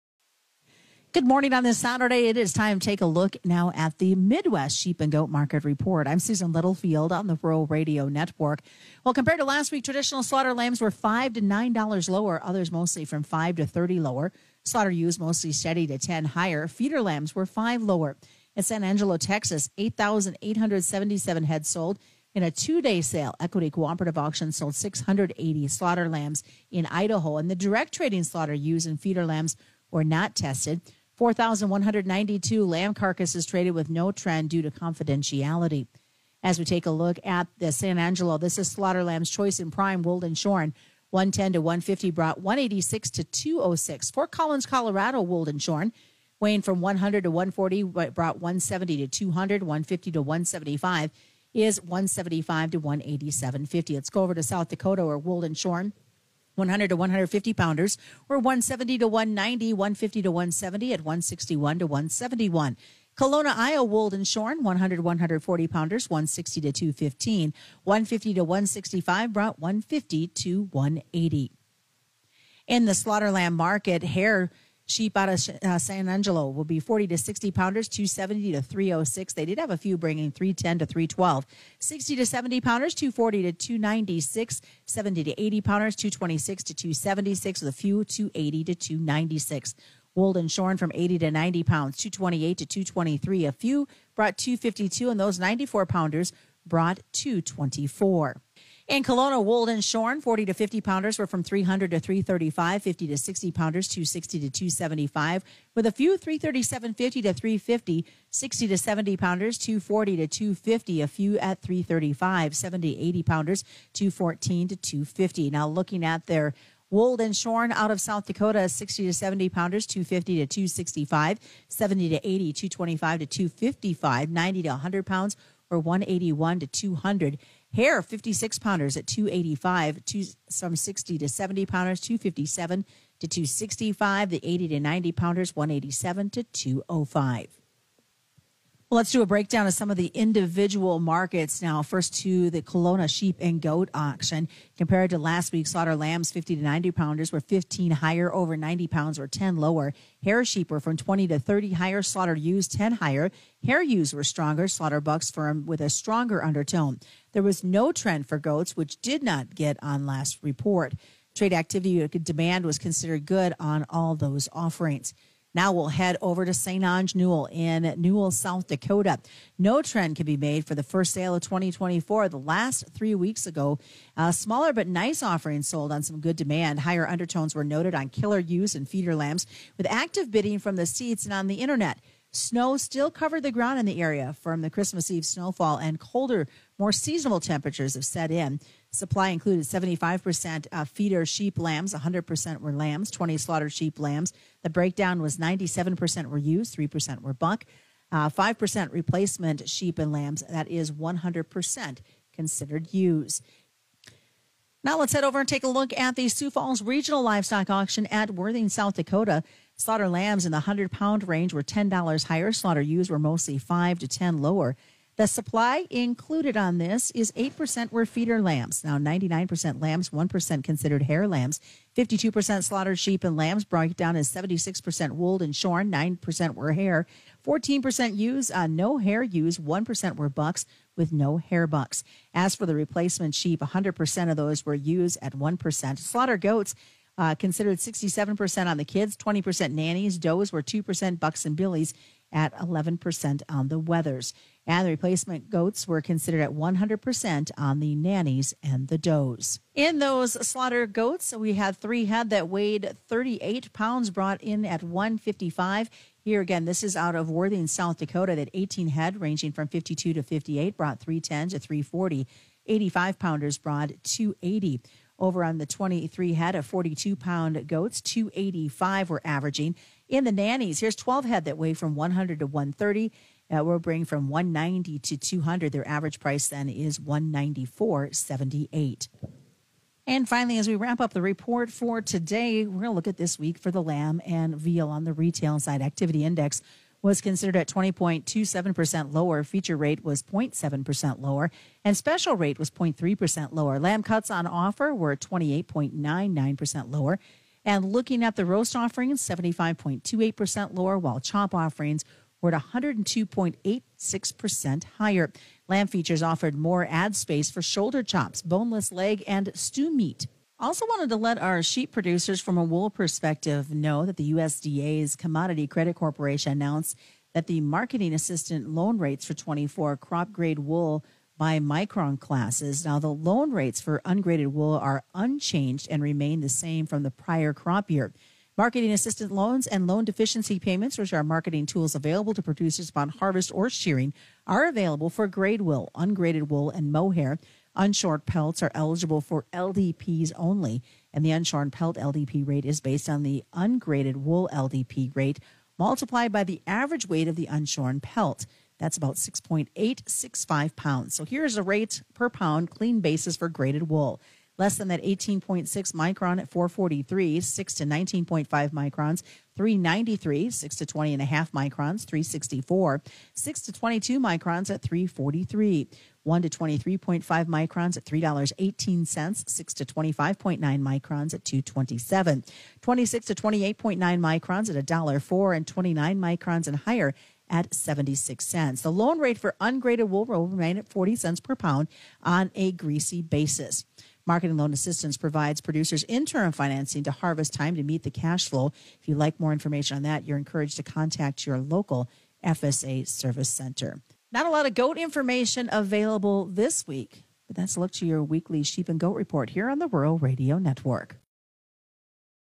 Agriculture Feature Stories/Interviews
A run down of the midwest sheep & goat prices for the week that ended 1-5-24